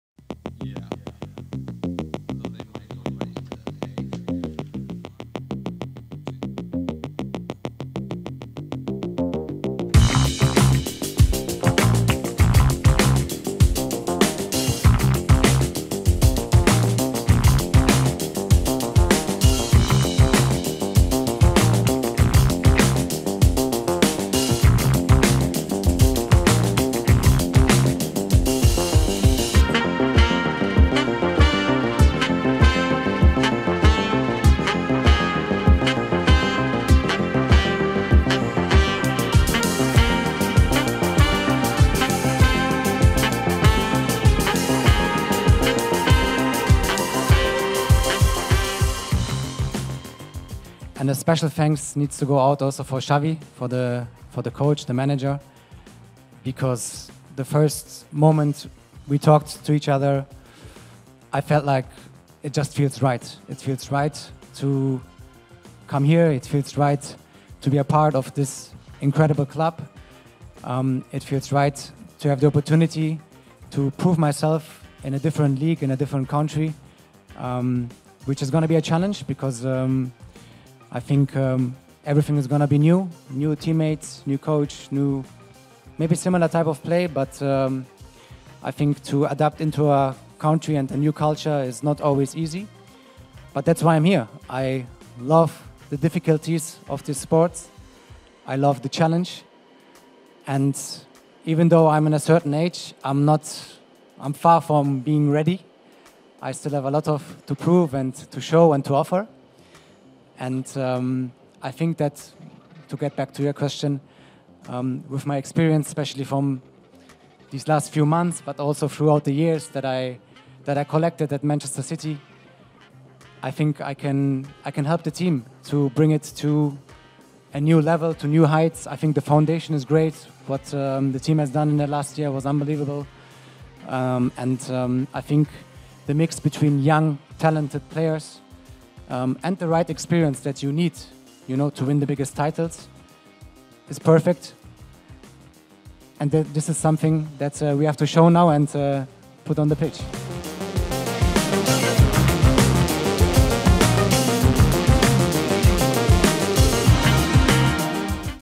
Ilkay Gundogan Yakin Bersama Barcelona dapat Menggapai Gelar Juara
Berita Video, pada acara perkenalan bersama Barcelona, Ilkay Gundogan membagikan  pengalaman barunya bergabung di klub baru.